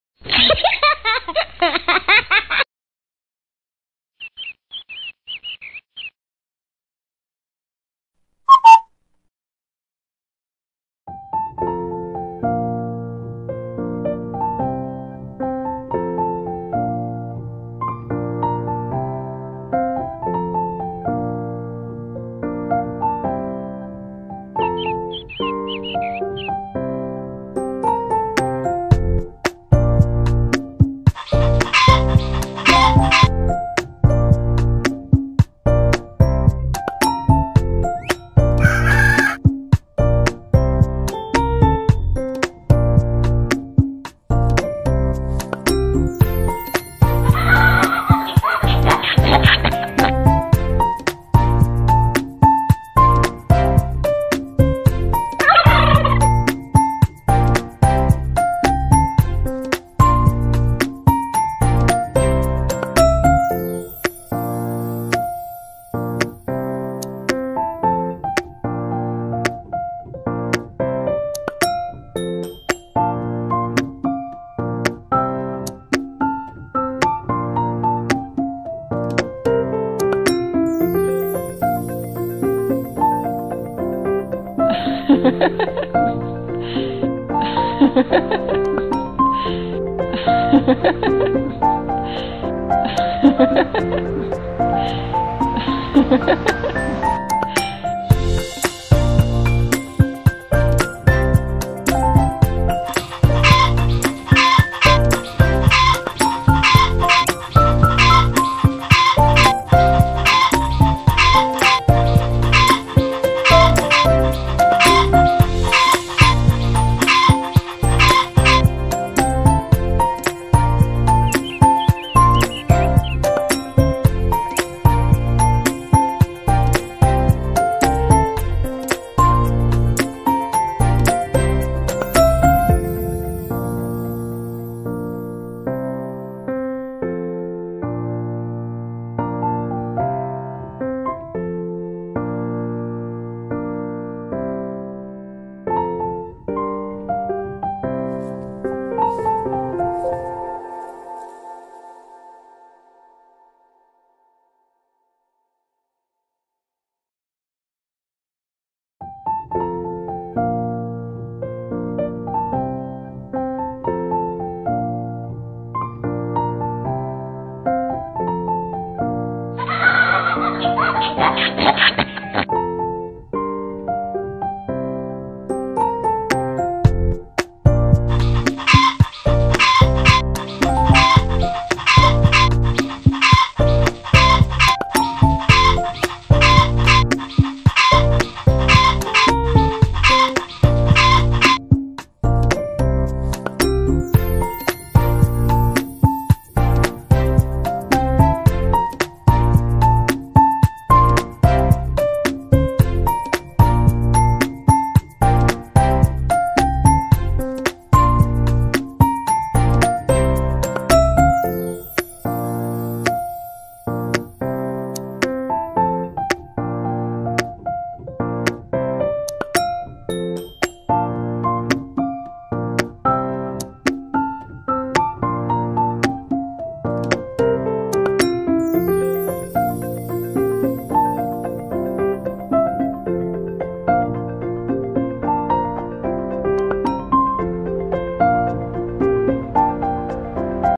Funny animal videos 😃 | Funniest animals 2023
I hope you enjoy this funny chicken dance,enjoy watching